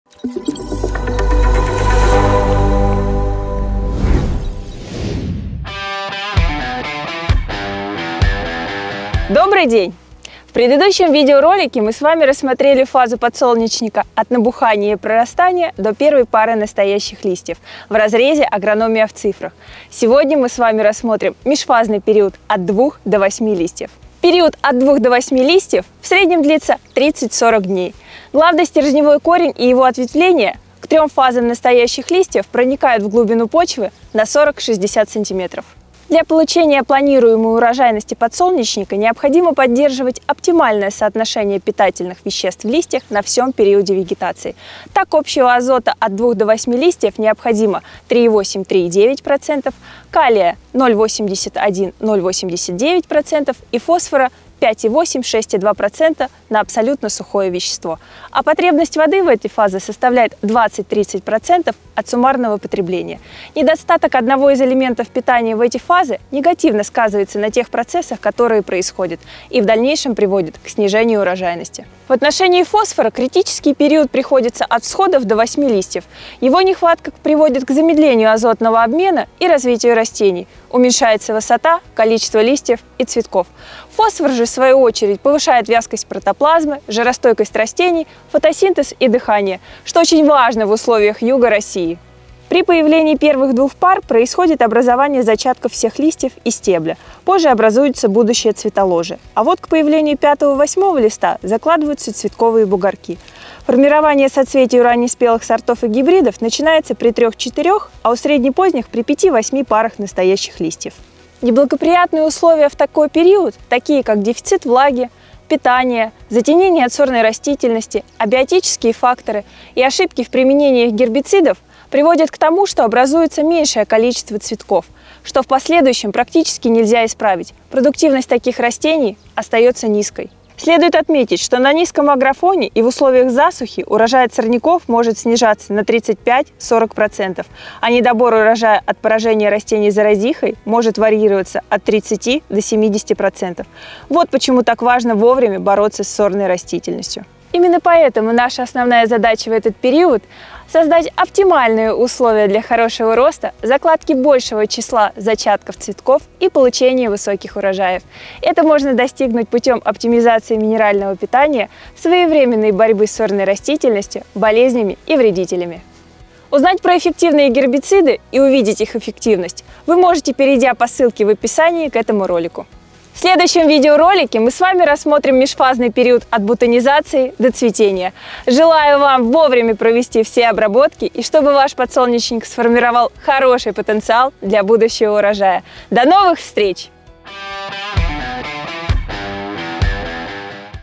Подсолнечник, фазы от 2-х до 8-ми листьев. "Агрономия в цифрах". Выпуск 2
В этом выпуске рассмотрен межфазный период от 2-х до 8-ми листьев в разрезе "Агрономия в цифрах". Отображены потребности подсолнечника, процессы происходящие в растениях и факторы влияющие на урожайность.

Наша основная задача в этот период – создать растениям оптимальные условия, для хорошего роста, способствующие закладке большего числа зачатков цветков и получению высоких урожаев.

В этом нам помогут гербициды ЛИСТЕГО Про и КАПТОРА Плюс, применяемые на гибридах подсолнечника производственной системы Clearfield Plus, а также гербицид КАПТОРА для системы Clearfield.